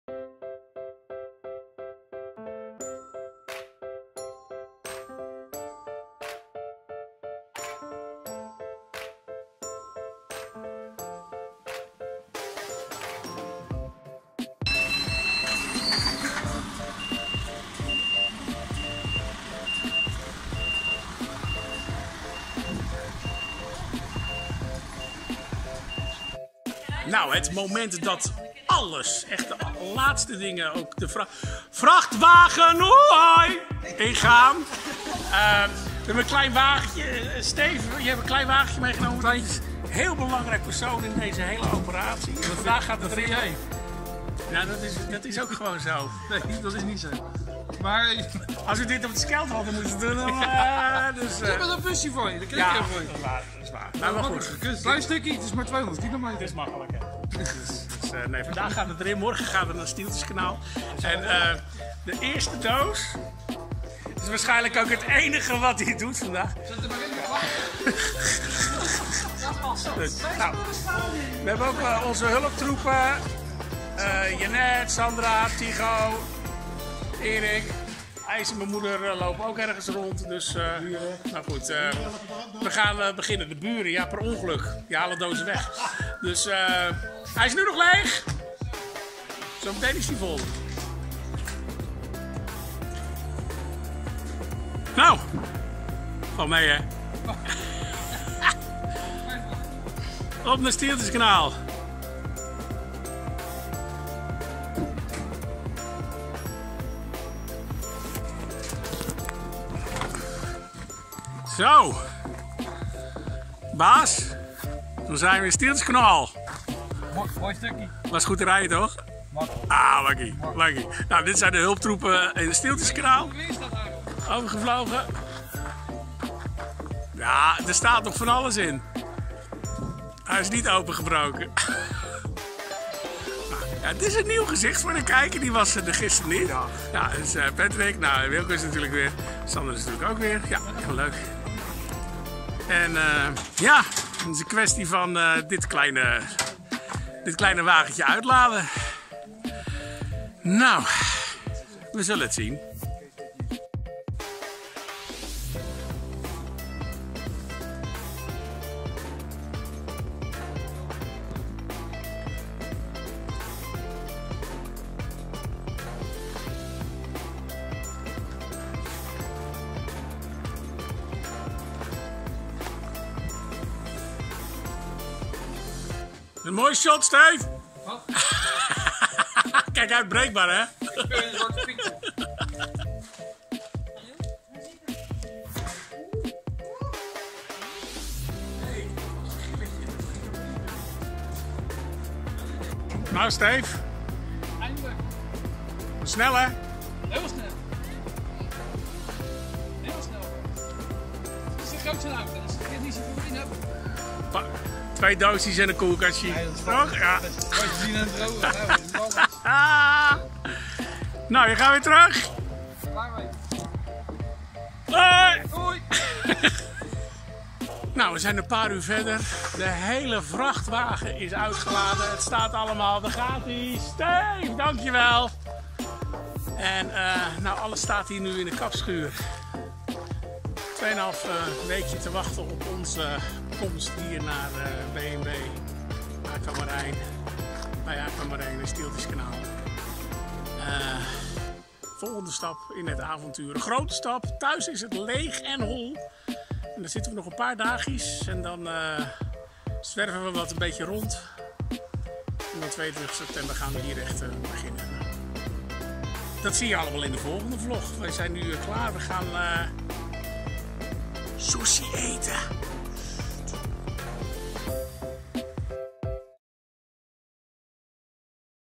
Van Katwijk naar Drenthe (3)
Onze verhuizing van Katwijk aan Zee naar Stieltjeskanaal in Drenthe om daar een geweldig bedrijf met  B&B, Mini-Camping en Vakantie- en Hotelhuisjes over te nemen.